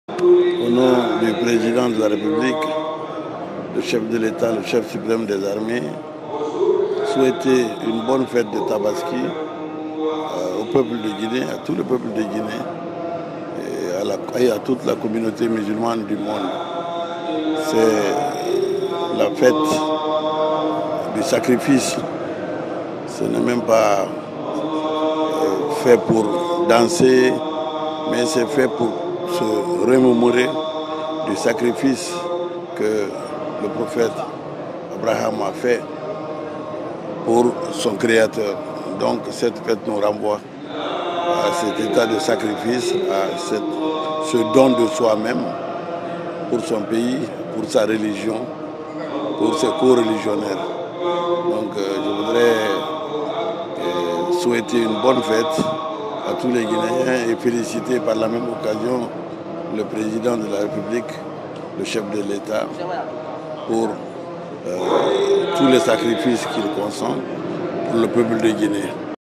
Morissanda Kouyaté: « cette fête (la Tabaski) nous renvoie au sacrifice, au don de soi pour son pays
C’est à la mosquée Fayçal de Conakry que Dr Morissanda Kouyaté, ministre des Affaires Étrangères, de l’intégration africaine et des Guinéens de l’étranger, a effectué la prière de l’Aïd El Kebir (ou fête de tabaski) ce mercredi, 28 juin 2023. Le chef de la diplomatie guinéenne a mis l’occasion à profit pour adresser un message à ses compatriotes, tout en leur rappelant l’importance de cette journée de sacrifice et de pardon.